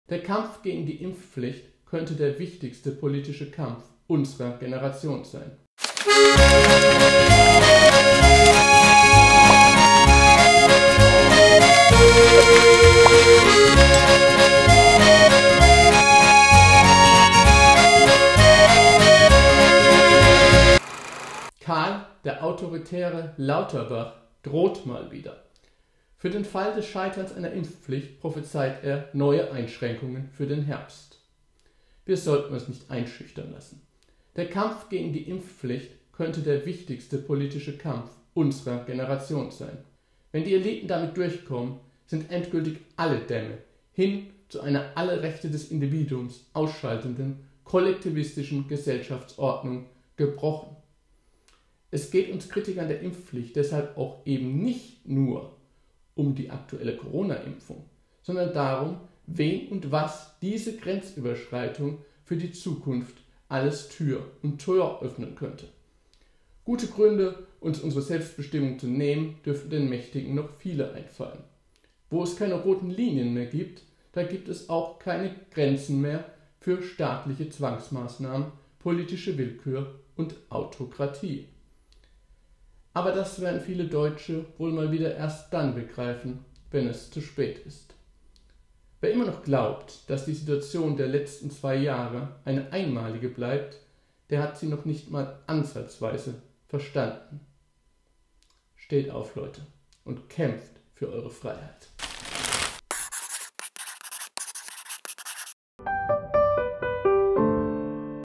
Corona: Karl "Der Autoritäre" Lauterbach droht mal wieder
Für den Fall eines Scheiterns der Impfpflicht droht Bundesgesundheitsminister Karl Lauterbach (SPD) mit erneuten Corona-Zwangsmaßnahmen ab dem Herbst.

Warum wir uns davon nicht einschüchtern lassen dürfen
und der Kampf gegen die totalitäre Impfpflicht der wichtigste politische Kampf unserer Generation sein könnte, erläutert DeutschlandKURIER🇩🇪-Kolumnist Boris T. Kaiser.